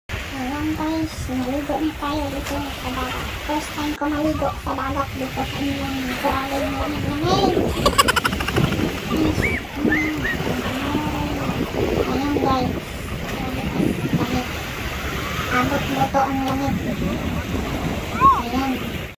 LIGO CHALLENGE IN COLD ❄️ BEACH ENGLAND
Family bonding at Danes Dyke Beach and go swimming this cold beach but but not so bad after all. Nice swimming in cold water.